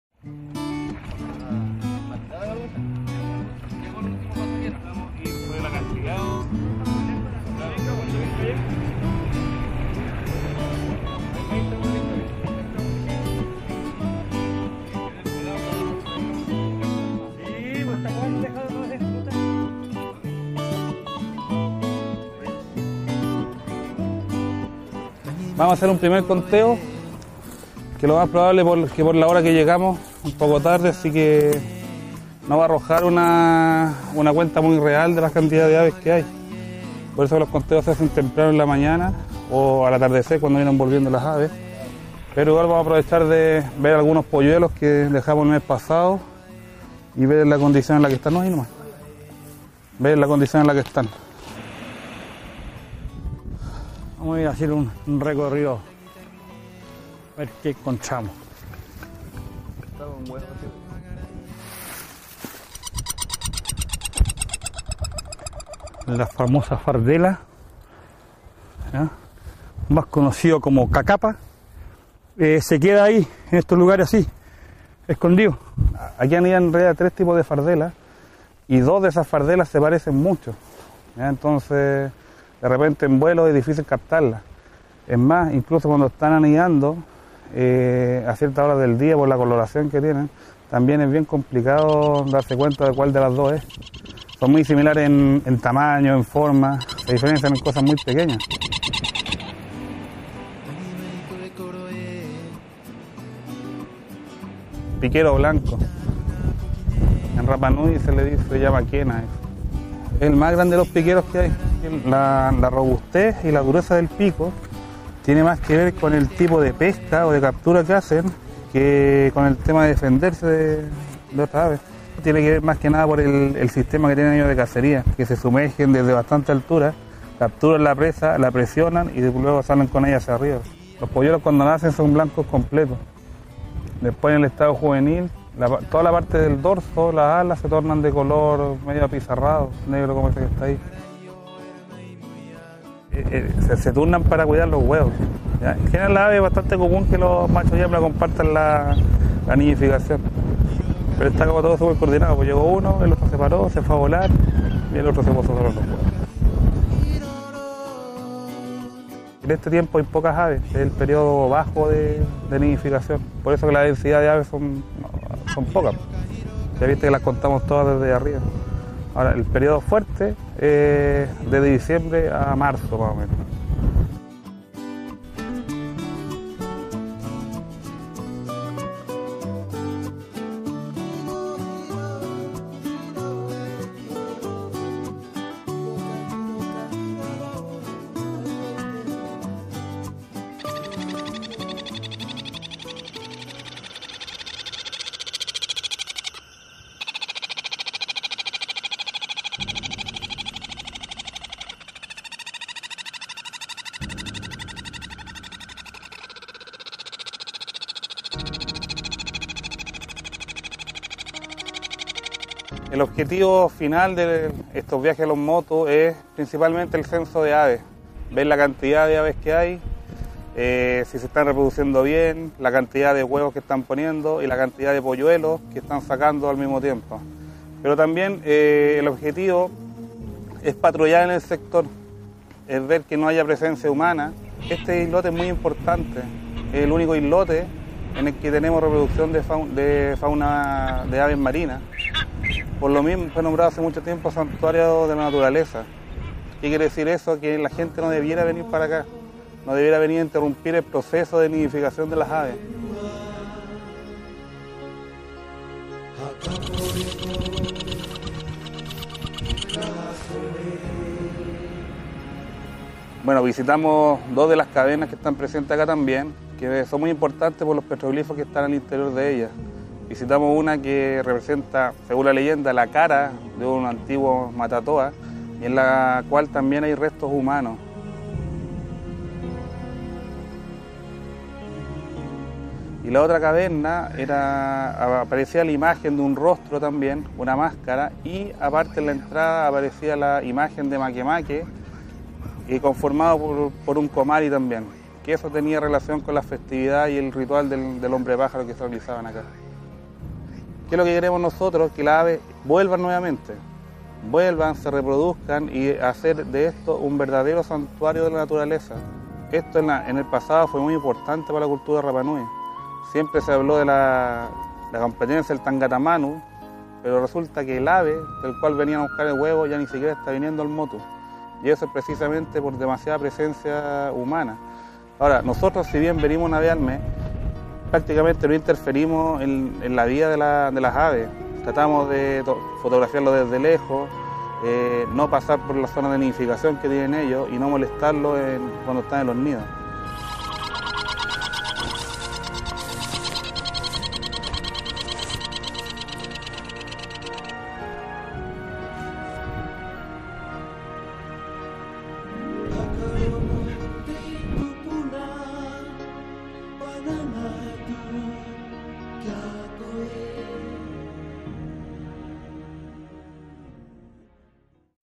Monitoreo de avifauna nativa
Reportaje de Semilla Films acerca de las labores de censo de avifauna y monitoreo de las áreas de reproducción de avifauna en el Parque Nacional Rapa Nui (2010).